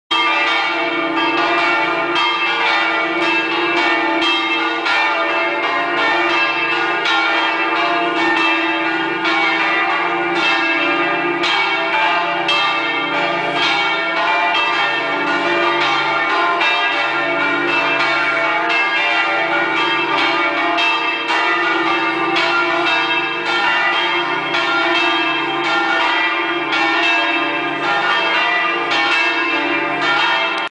Glocken der Antoniusbasilika in Padua (Norditalien)
Das Geläute im Südturm der Antoniusbasilika in Padua (Tonfolge: d'-e'-f'-g'). Ob es noch mehr als diese vier Glocken gibt, ist mir nicht bekannt.